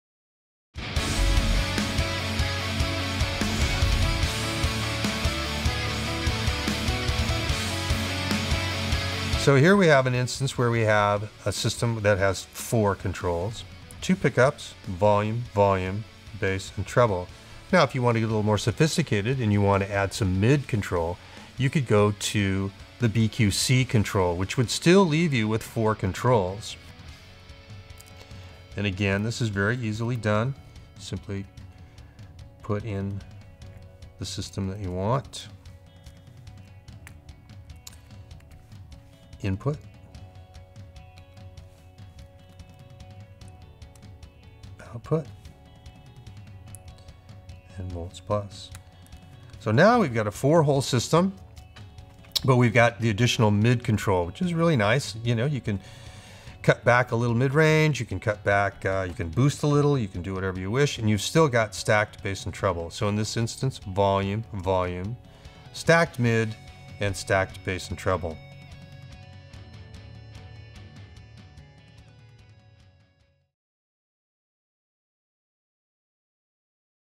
How to install EMG BQC system for bass
Rob Turner shows you how to install EMG BQC system for bass using EMG's solderless system.